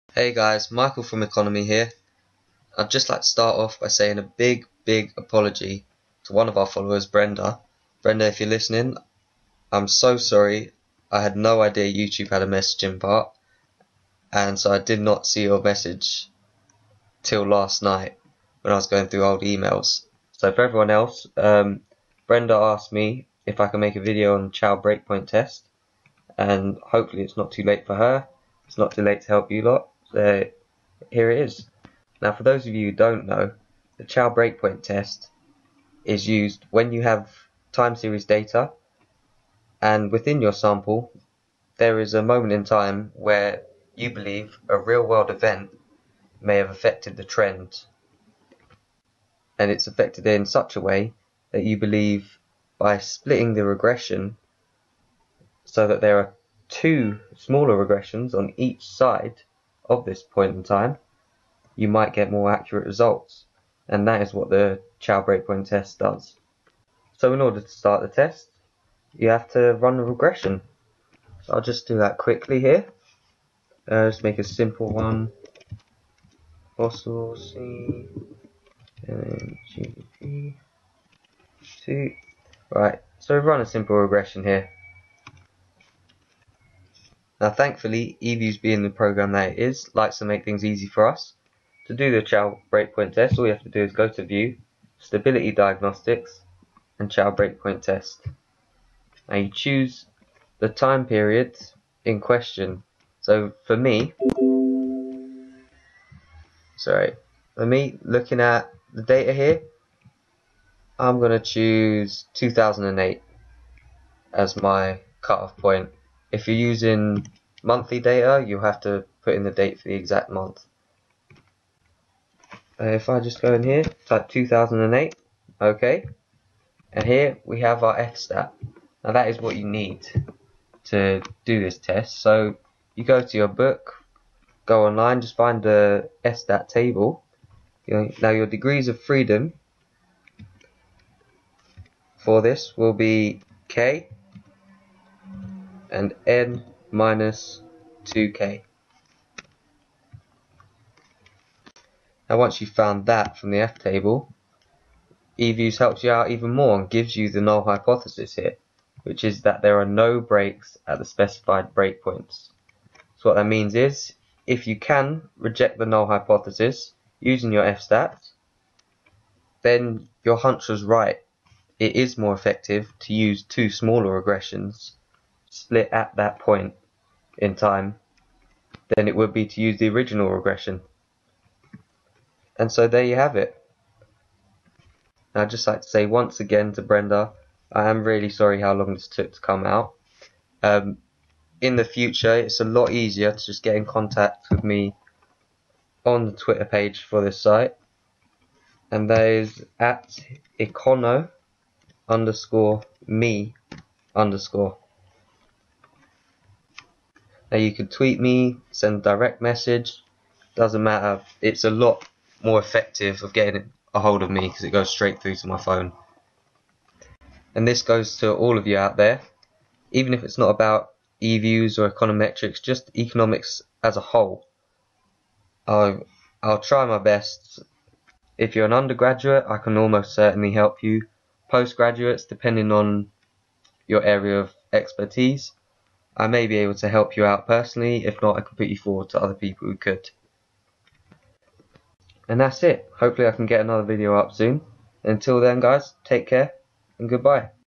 EViews Tutorial - Chow Breakpoint Test
How to do a Chow Breakpoint Test using Eviews